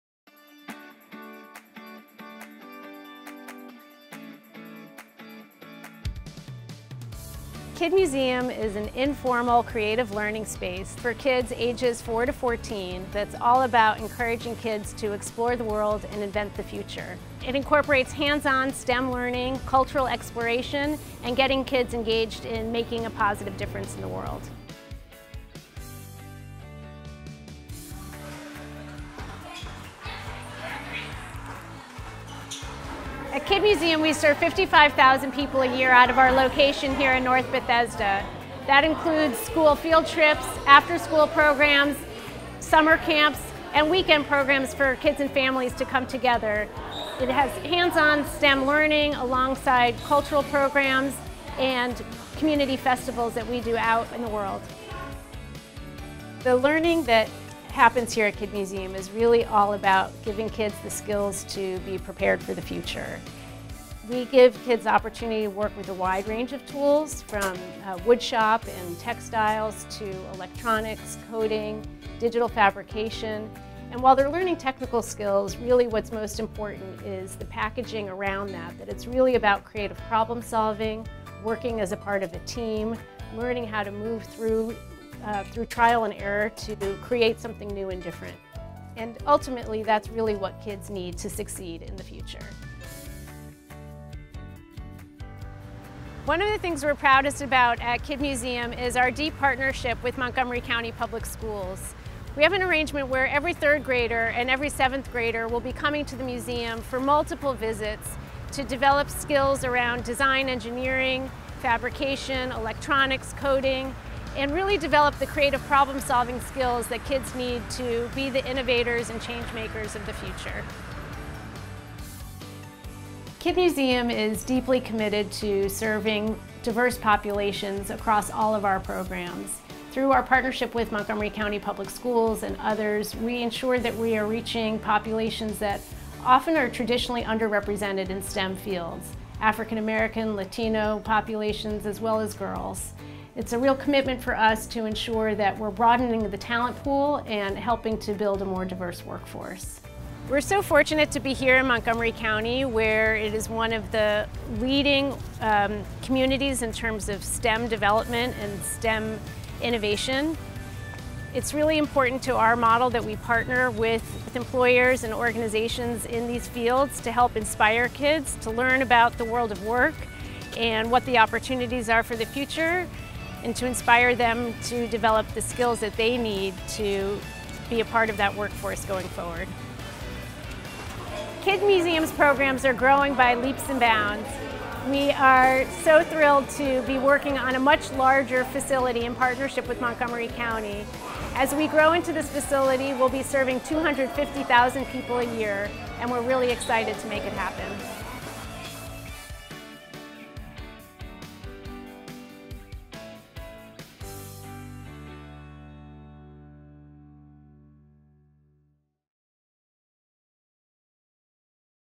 Think Inspiration: KID Museum
Cara Lesser, Founder and Executive Director of KID Museum, explains how her organization teaches young minds to explore the world and invent the future through hands-on STEM learning.